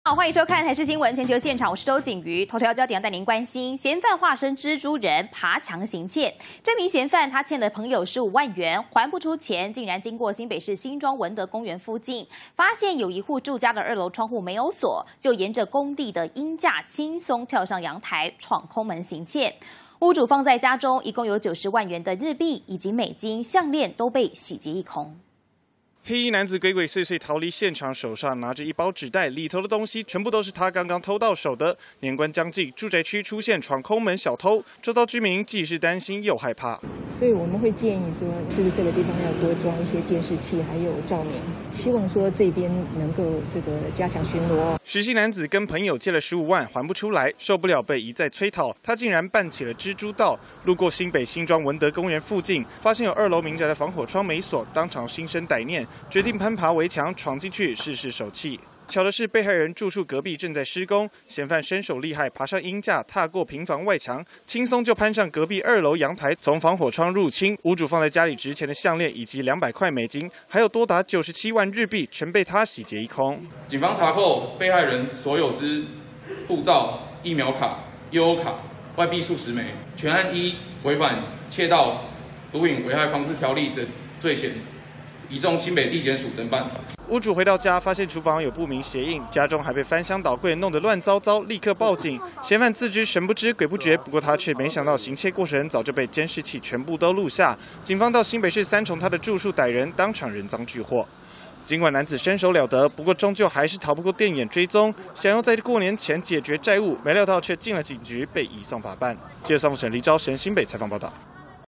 欠錢還不出 男子化身蜘蛛大盜偷來還
男子化身蜘蛛人爬牆行竊！ 嫌犯欠朋友15萬，一直還不出來，竟然去偷別人的錢要來還債，他經過新北市新莊文德公園附近，發現有工地旁邊二樓住家窗戶沒鎖，就沿著鷹架輕鬆跳上陽台，闖空門行竊，屋主放在家裡97萬日幣，還有美金和項鍊，全被洗劫一空。警方透過電眼追蹤成功逮到人，得手的贓款也真的被他拿去還債了。